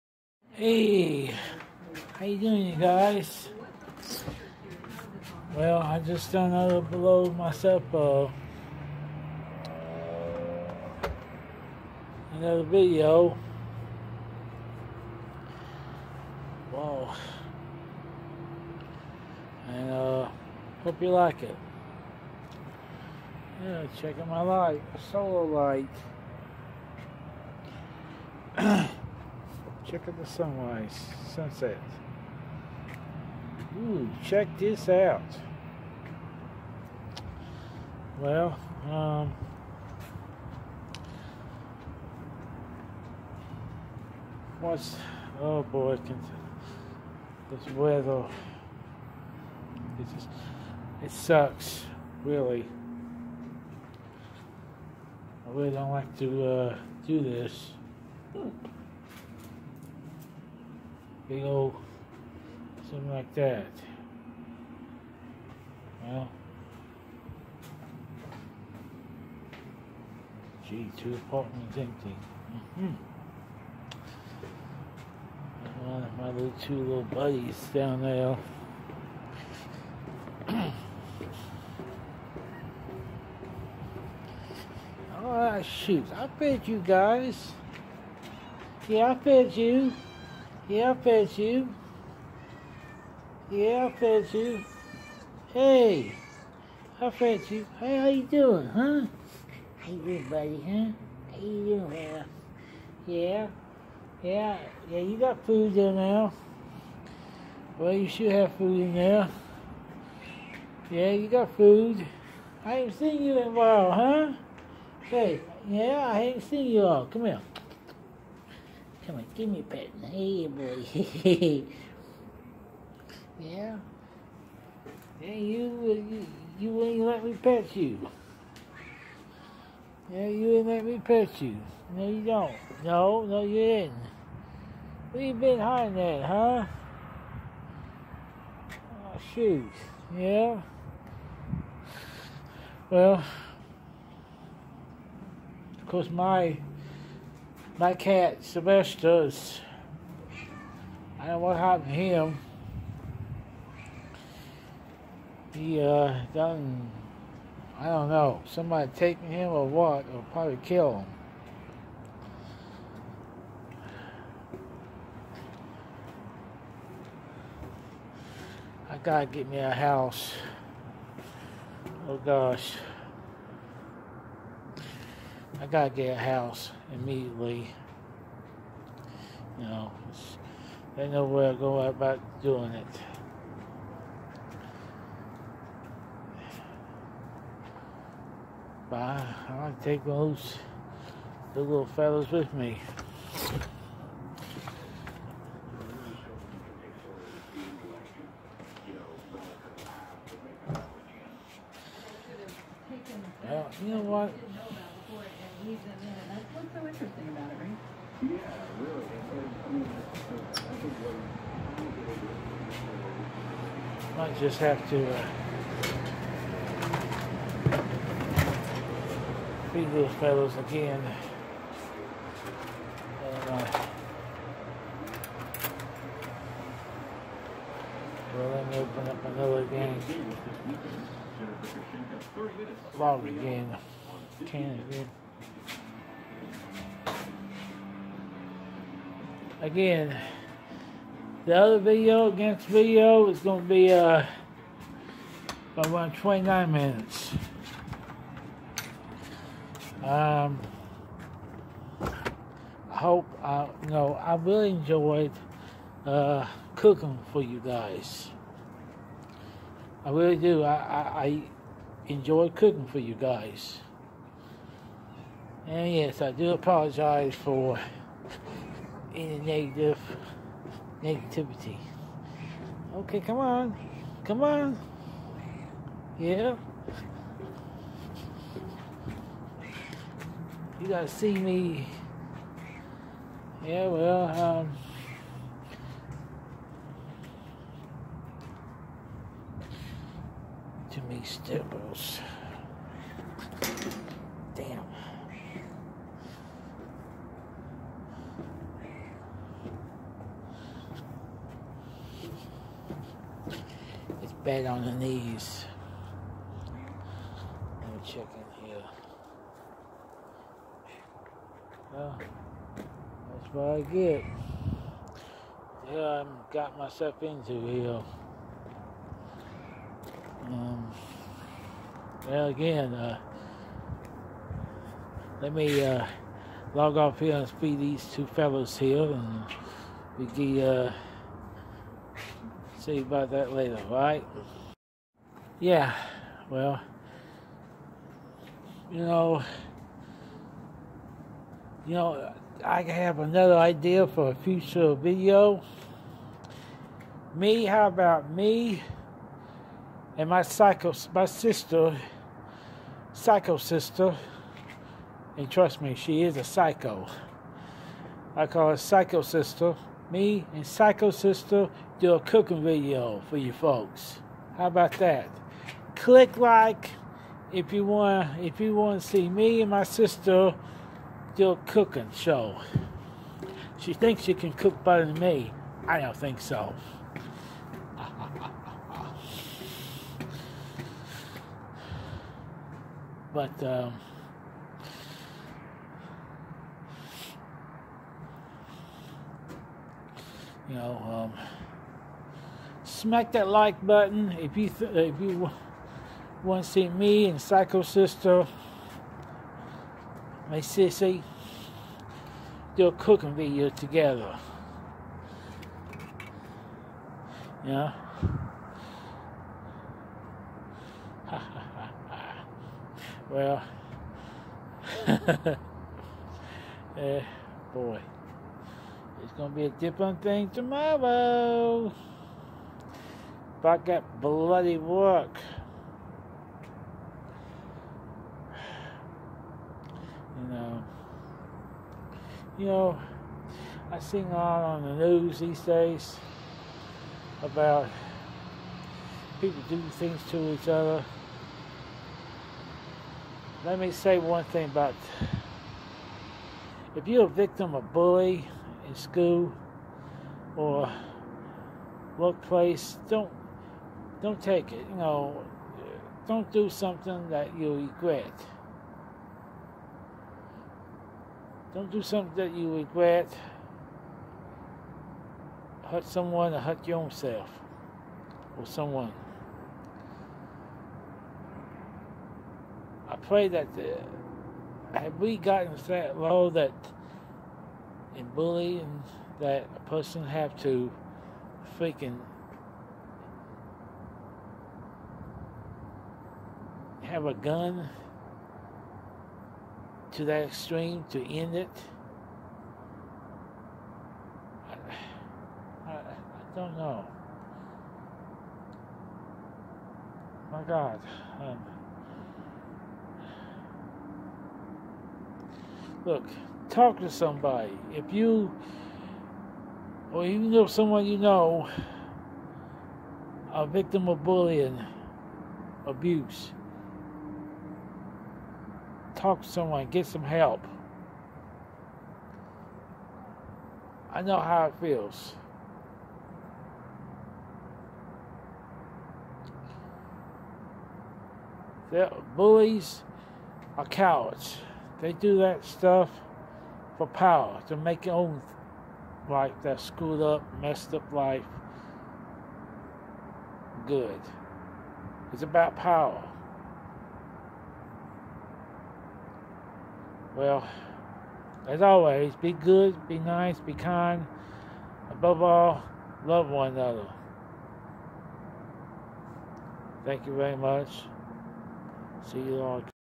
My Wednesday Daily Blog's..
Feeding The Cats and Stop the Bullying and Get Help theres Hope..Futre Video's..